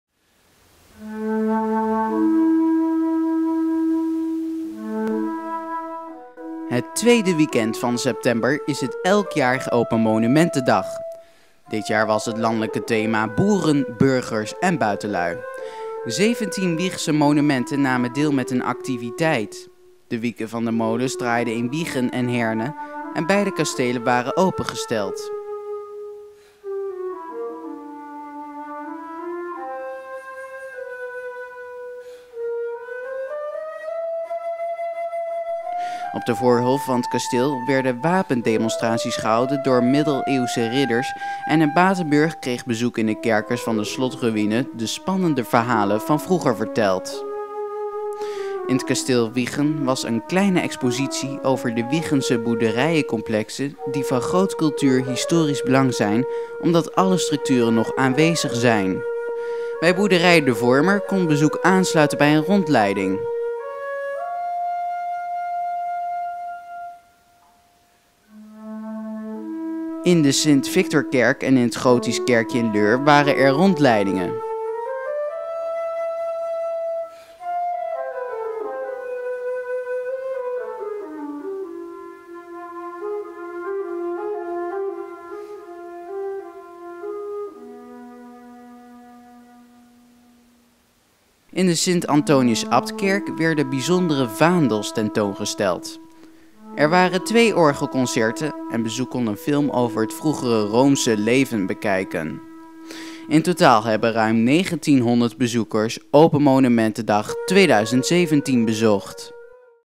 Open Monumentendag Wijchen - Nieuws
In het weekend van 9 en 10 september was het weer open monumentenweekend. Op de voorhof van het kasteel werden riddertoernooien gehouden waar ridders in echte harnassen en met gevaarlijke zwaarden streden. Kinderen werden betrokken middels plastic gevechtsgereedschap.
In de raadzaal van het kasteel was een expositie in het thema.
Diverse monumenten in Wijchen en in onze kerndorpen werden opengesteld.